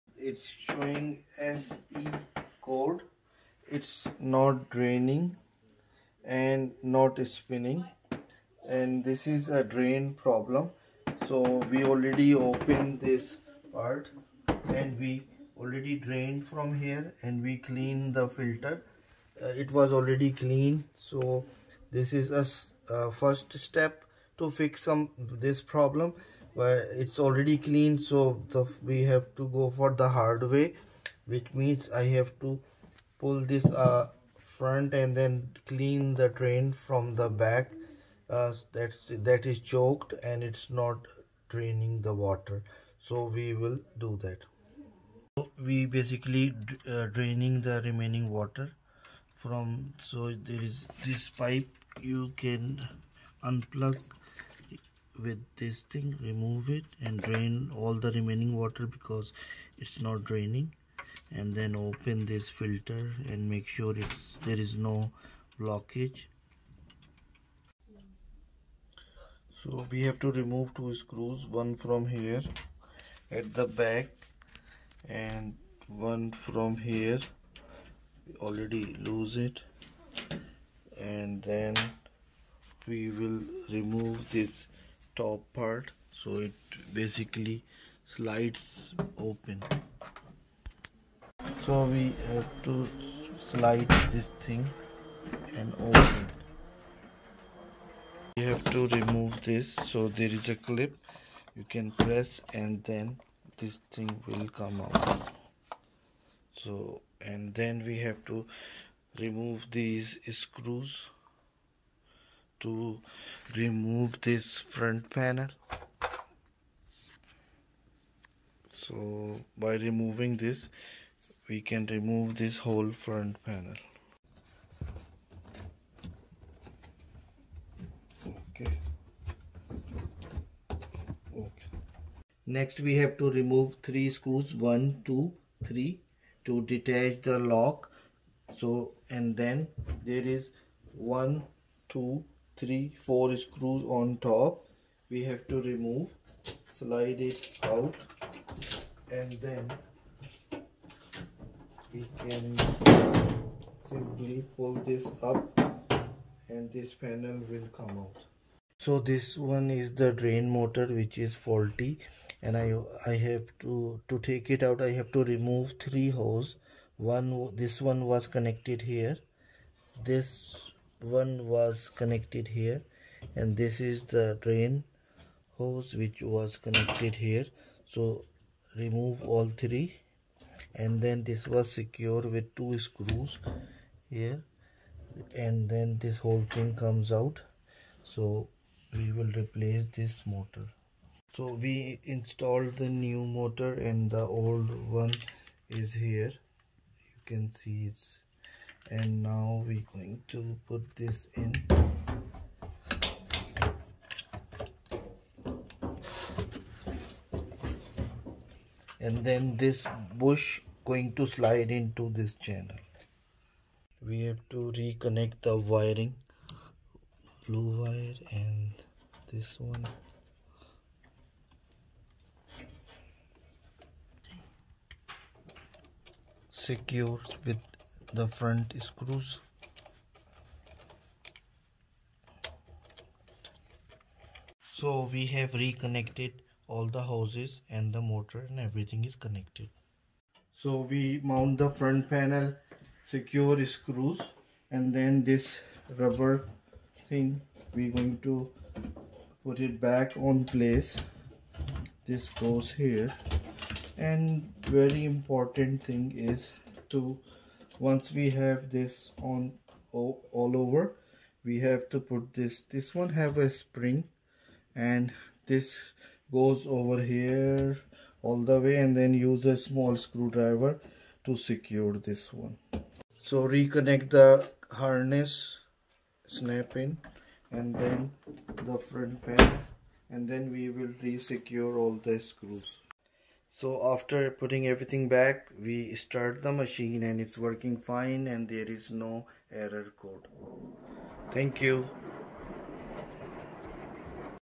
Samsung washing machine SE Error // How to Replace Drain Motor
How to replace Drain Motor Step by Step